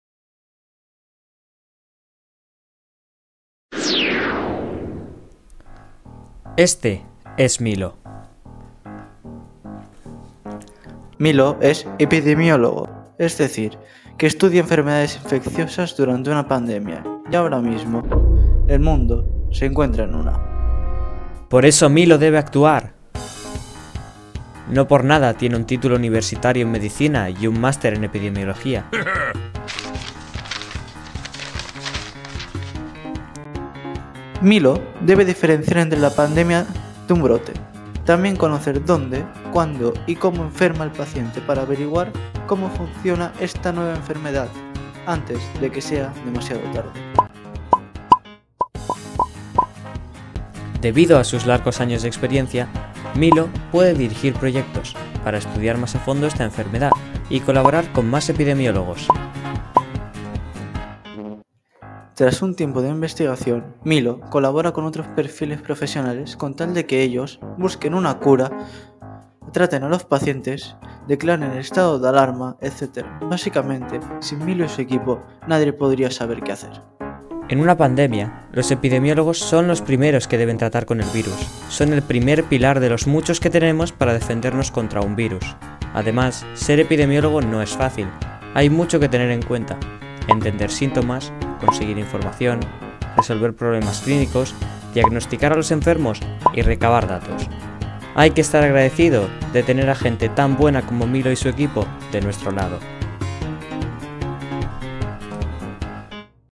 Milo, el epidemiólogo [1r Premio Science4Pandemics]
De los 18 participantes en la Unidad Didáctica “Descubriendo los perfiles profesionales implicados en la gestión de una pandemia” de Science4Pandemics, el vídeo “Milo, el epidemiólogo” recibió el Primer Premio.

Autores: Jordi Colomo y Narcís Rabasedas
Escuela y curso: Vedruna Palamós 4º ESO (Girona, Catalunya)

Los videos fueron evaluados por 6 evaluadores miembros del consorcio
(ámbito científico, comunicación y educación). En el marco de evaluación se tuvieron en cuenta los siguientes aspectos:

- Precisión de la información en la presentación del profesional elegido
- Coherencia de la información y del género discursivo (adaptado al
propósito y al canal audiovisual)
- Vocabulario y lenguaje
- Creatividad y originalidad
- Uso de herramientas digitales